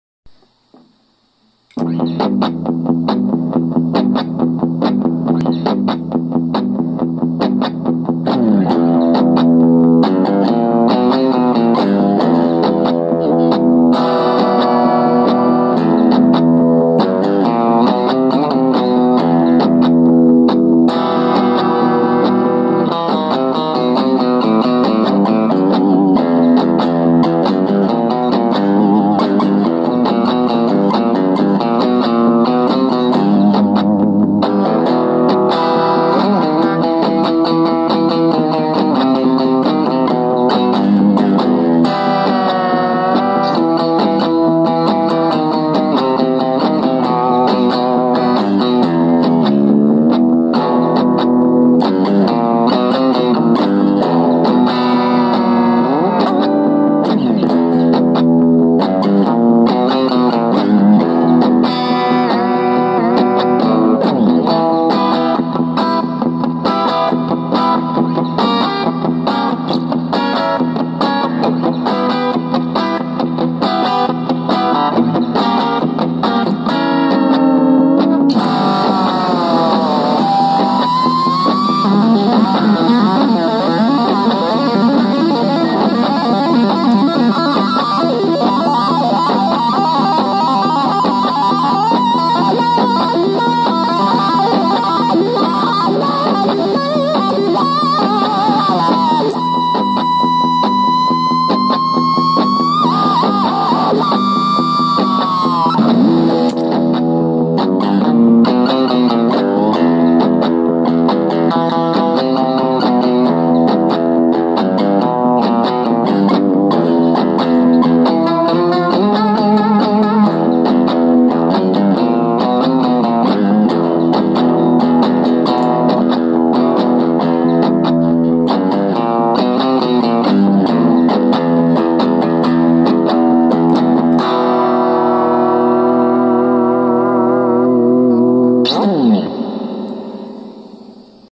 Creepy Surf Rock Guitar Song
Improvisation on Fender guitar - No studio recording, edits or processing. Video is uncut and straight off my phone. Amplifier is a 9V Pignose and all pedals are running off battery power.